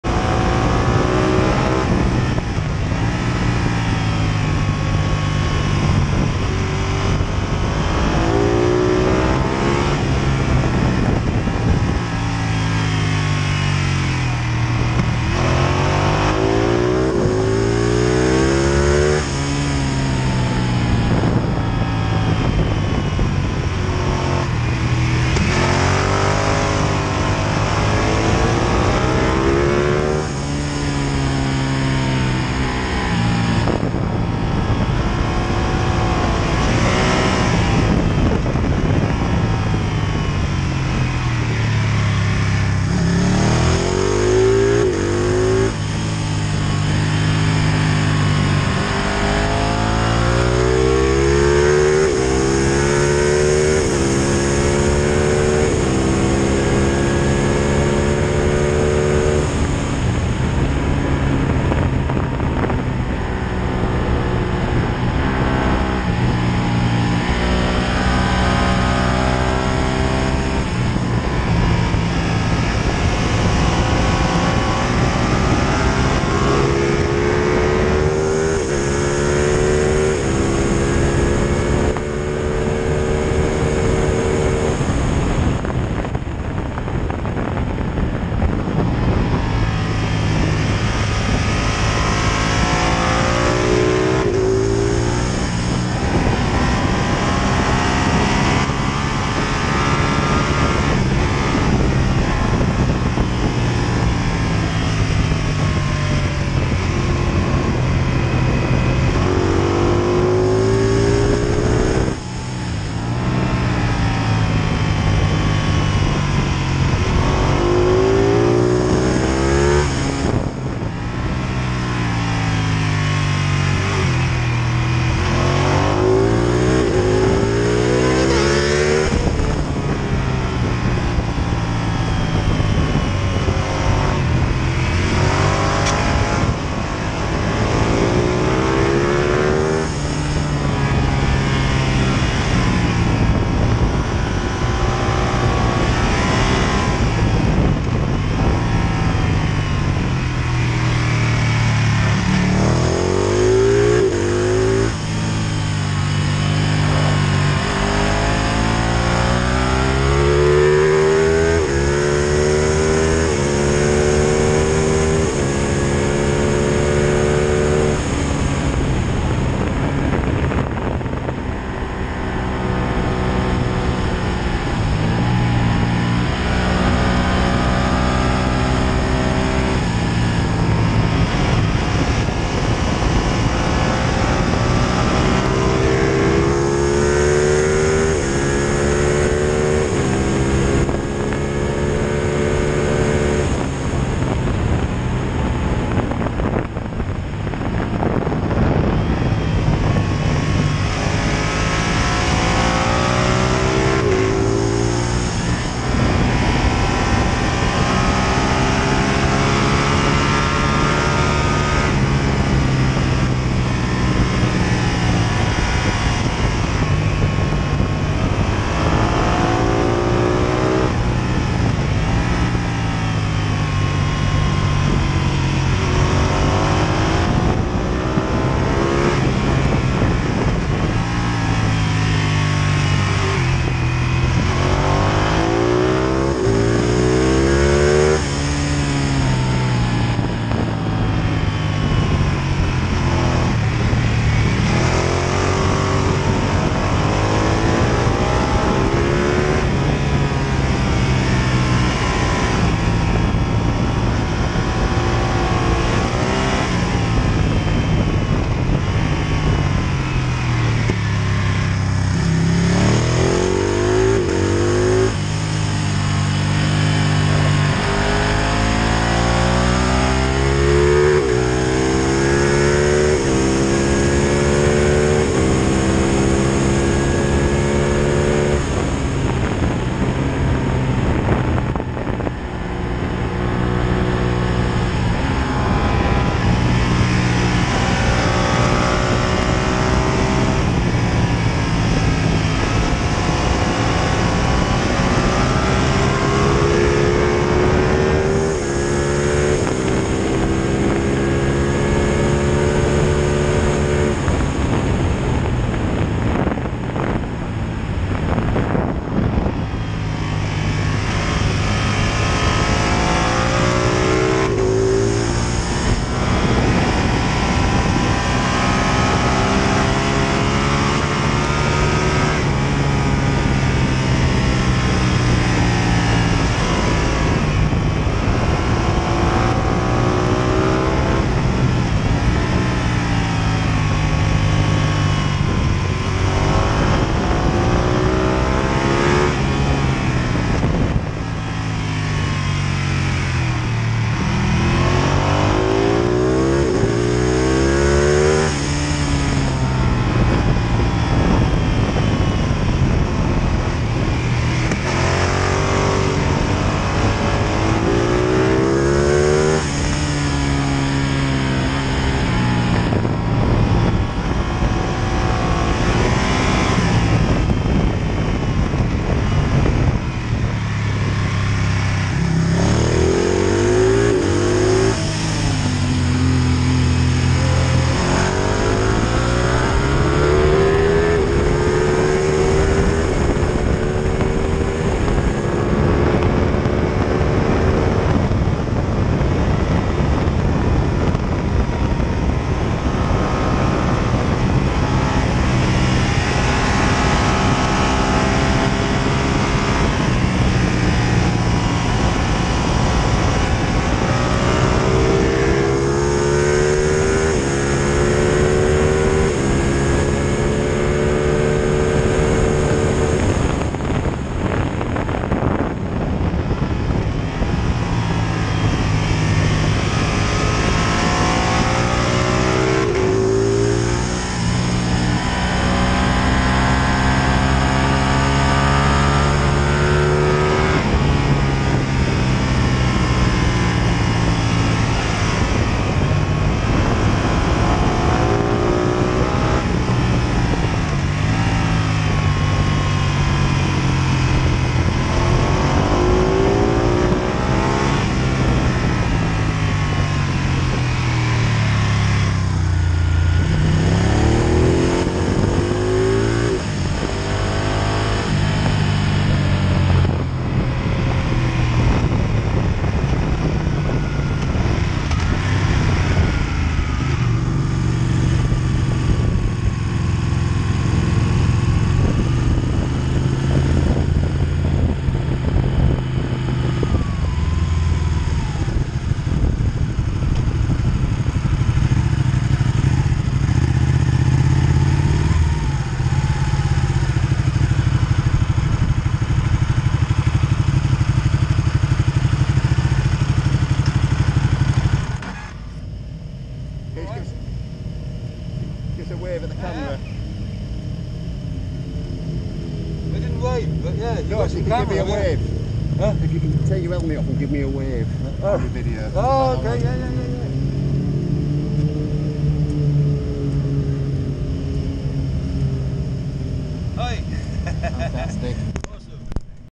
cadwell park on a JW track day on my ducati 899 29/09/21
cadwell park on a JW track day on my 899 ducati 29/09/21